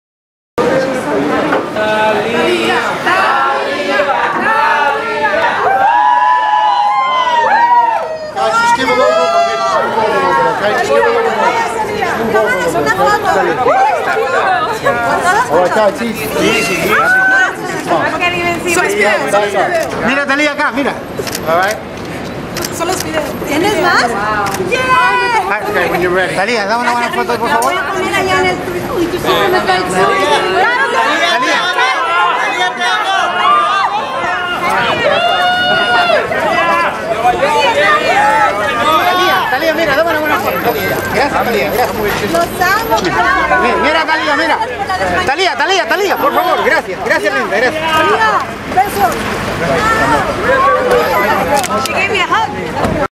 Thalia - Takes Photo with Fans at 'The Today Show' in NYC
Thalia stopped to pose with fans at 'The Today Show' in New York City. For authentic autographed memorabilia of many other celebrities, check out TopPix Autographs Get the Best Deals on Authentic Autographs on our eBay Store